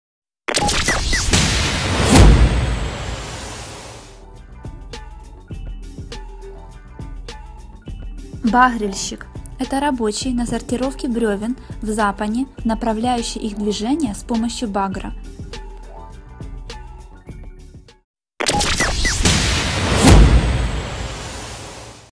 Багрильщик. Толковый Видеословарь русского языка
Что такое багрильщик. Дефиниция слова багрильщик. Багрильщик в Толковом Видеословаре русского языка.